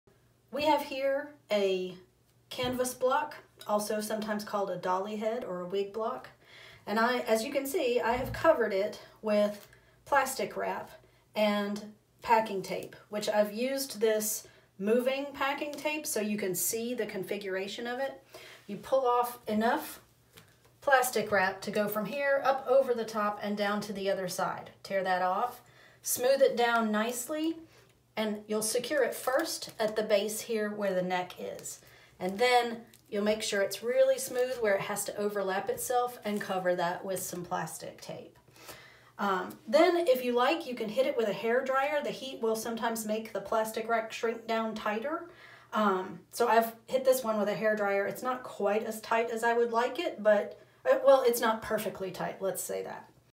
How to cover a canvas block with plastic wrap
In this clip, I'll show you how to cover canvas head form with plastic wrap and packing tape.

This will protect the canvas from dye transfer if you are working with black buckram or a bright colored material, and it also allows you to draw style lines on the head with a Sharpie marker for patterning or blocking buckram shapes.